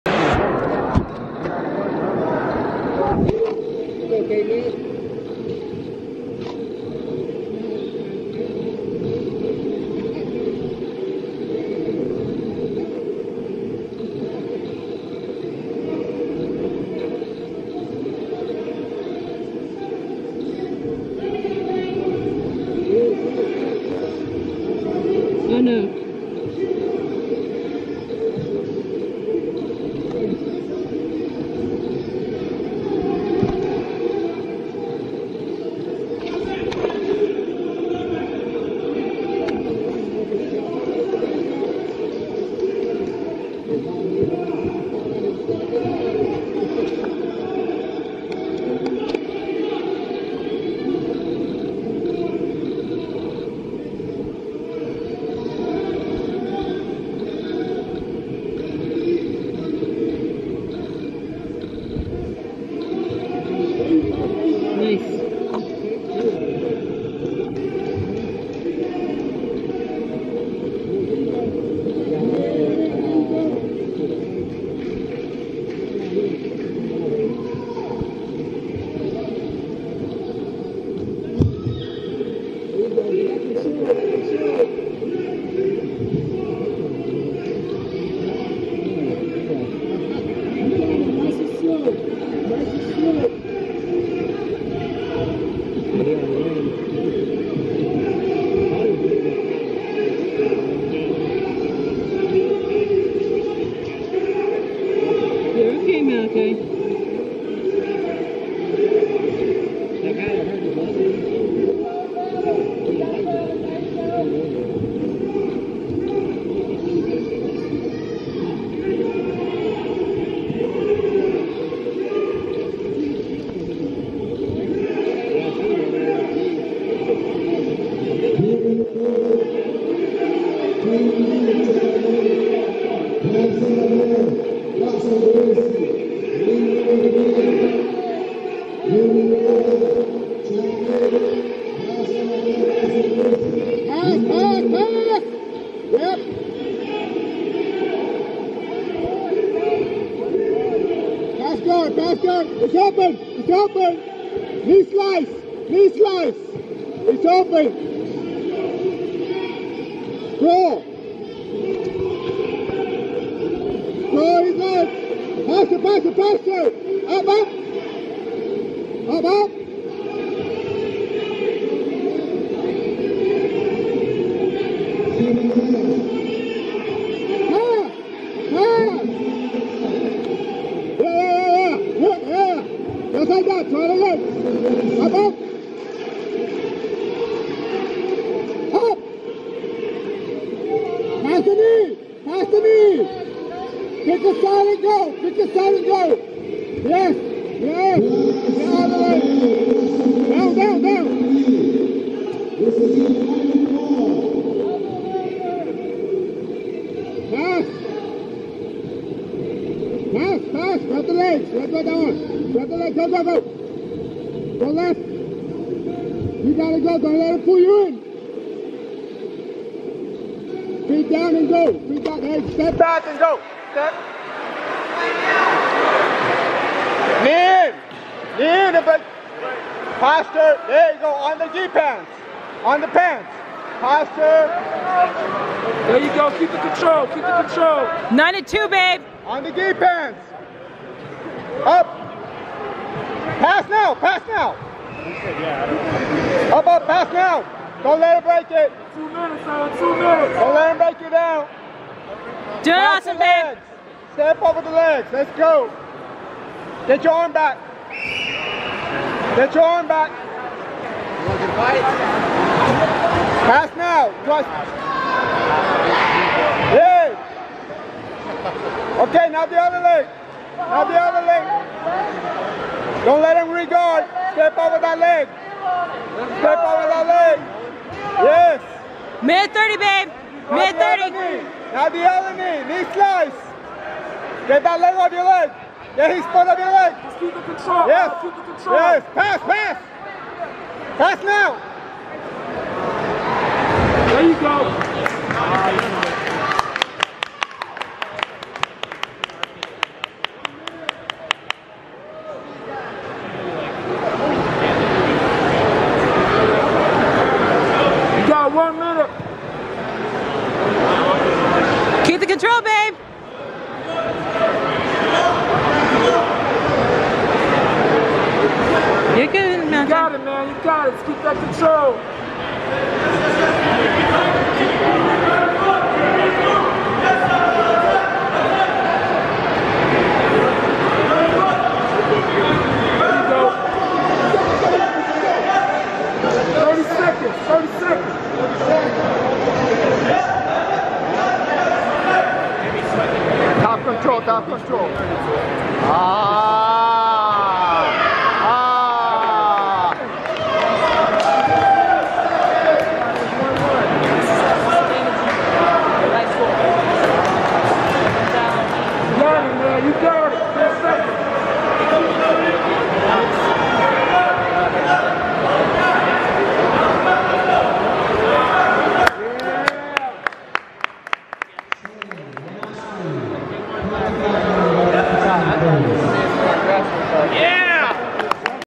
NAGA JAX 2017 NoGi Ultra Heavy Match
I went ahead and jumped up to Ultra Heavy Weight to take this guy on.  He didnt have an opponent so I volunteered to fill the spot.  He had 82lbs on me so my game plan was simple: dont stop moving.haha